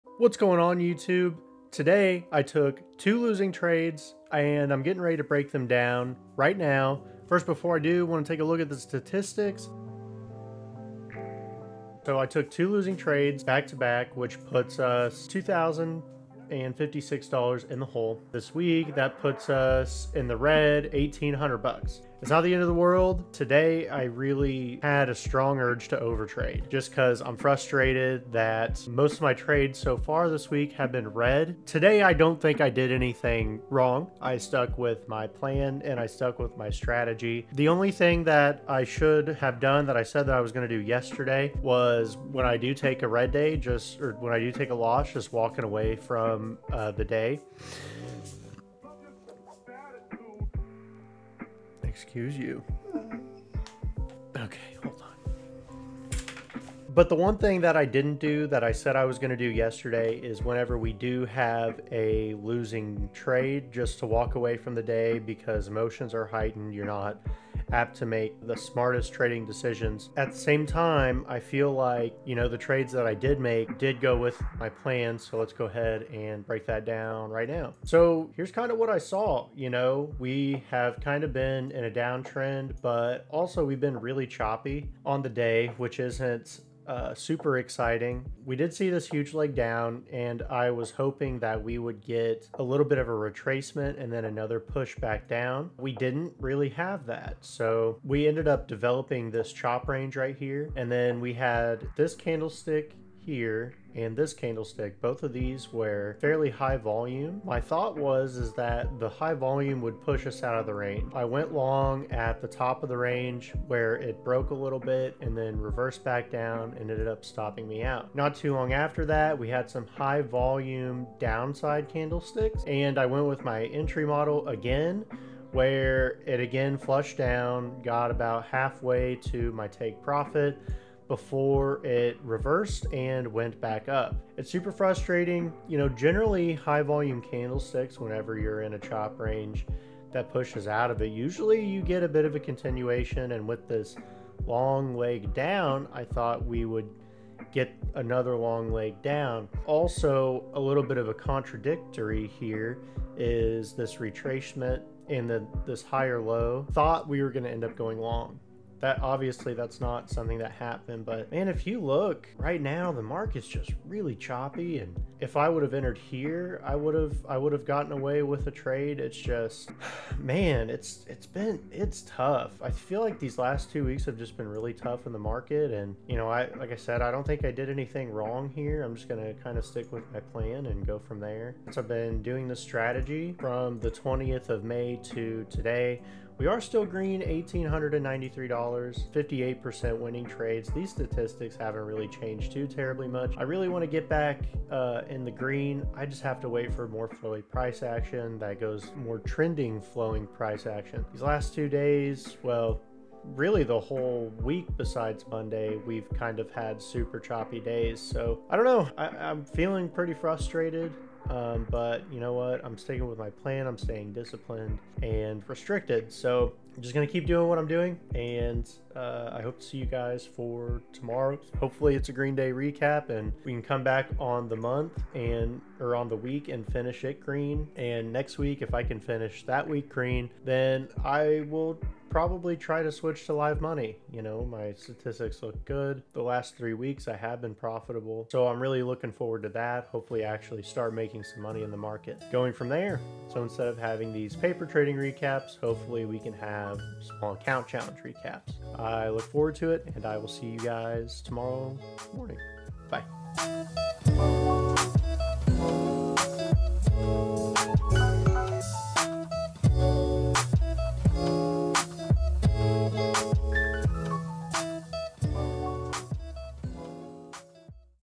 RED DAY RECAP the urge to overtrade is real | Day Trading Journal 6/6
Today I had 2 heavy losses in the market. But both trades followed my plan and my thinking behind them I feel is solid. Hopefully I can rebound the last day of the week. See you tomorrow!